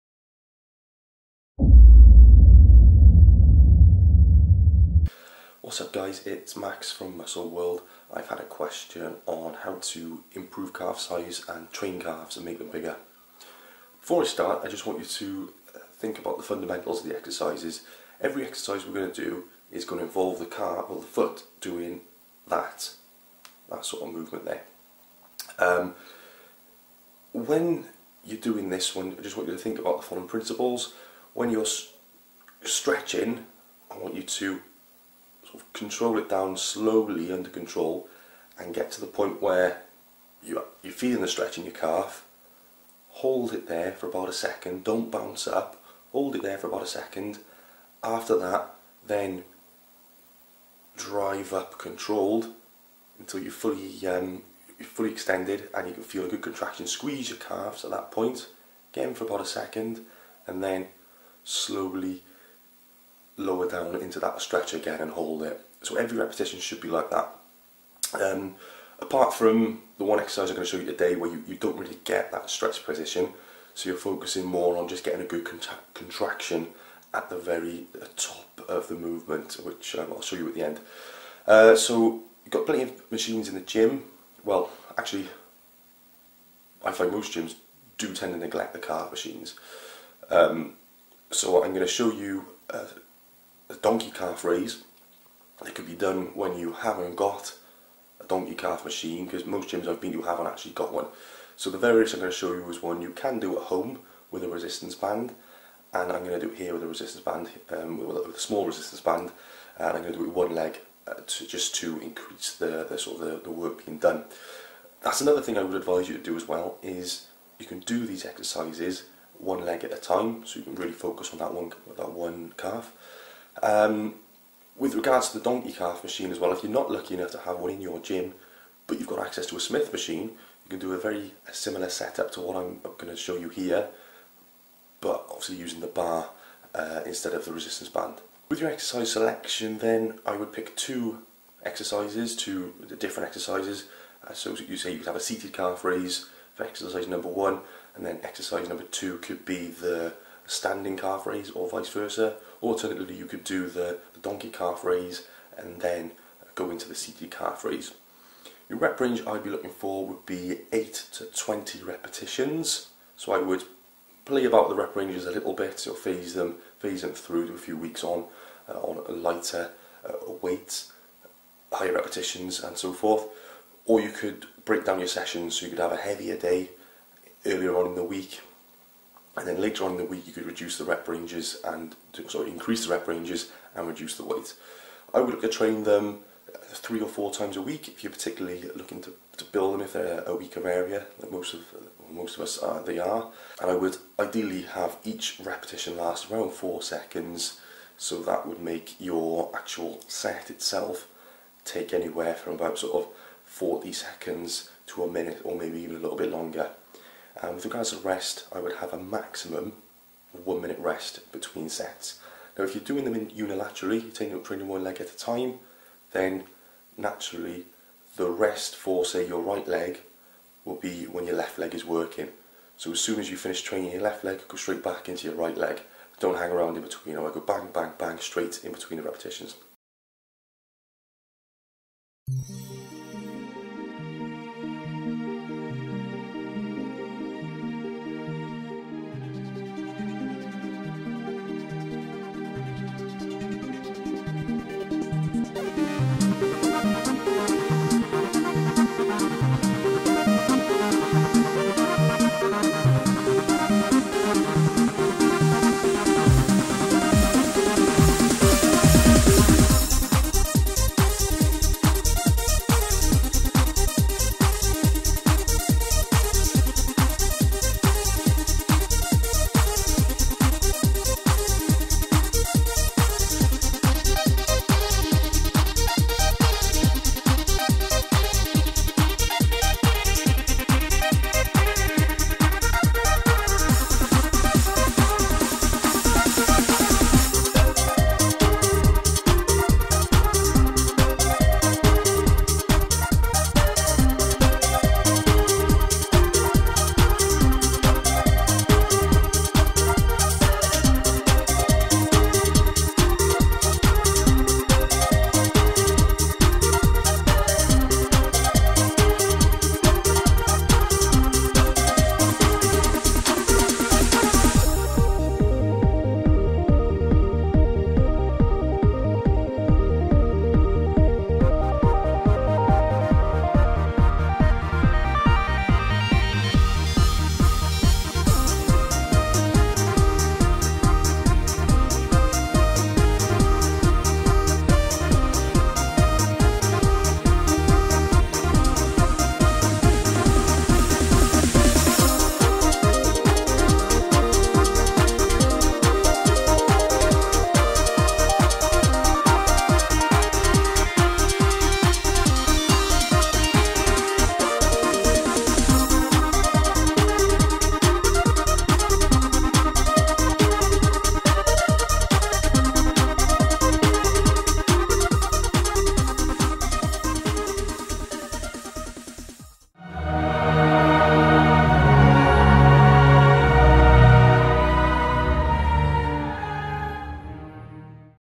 How to Build Calves | Train Calf Muscles Anywhere
Here a few exercises that can be done at the gym or at home.

Leg press calf extension
Donkey calf raise with bodyweight and resistance band
Squat calf raise